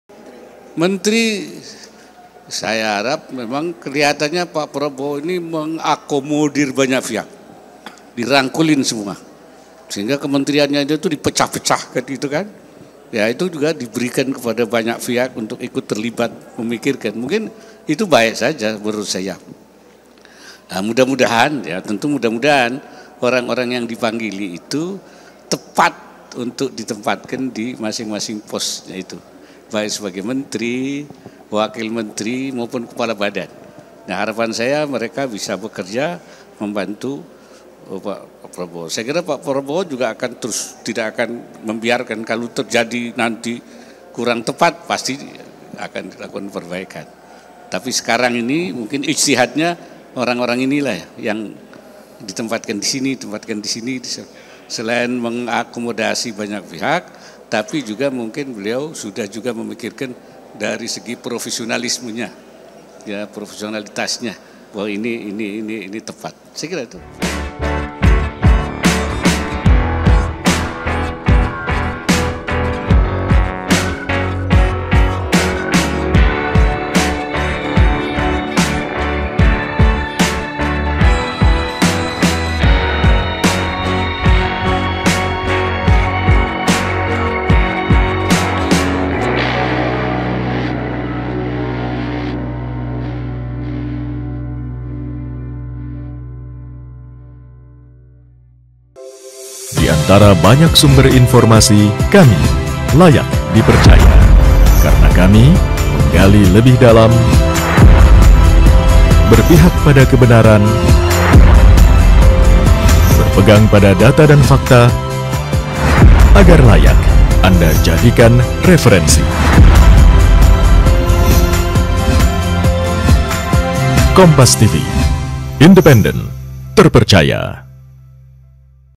Maruf Amin Terkait Calon Menteri Kabinet Prabowo-Gibran
INDRAMAYU, KOMPAS.TV  Wakil Presiden Maruf Amin berbicara soal para calon menteri dan calon wakil menteri kabinet Prabowo Subianto-Gibran Rakabuming.

Hal ini disampaikan Maruf Amin di Universitas Darul Ma'arif Indramayu pada Rabu (16/10/2024).

Maruf mengatakan bahwa Prabowo mengakomodasi banyak pihak.

"Mengenai menteri, saya harap memang kelihatannya Pak Prabowo ini mengakomodasi banyak pihak, dirangkul semua. Mungkin itu baik saja menurut saya," ujar Maruf. (*)

Kompas TV
Independen | Terpercaya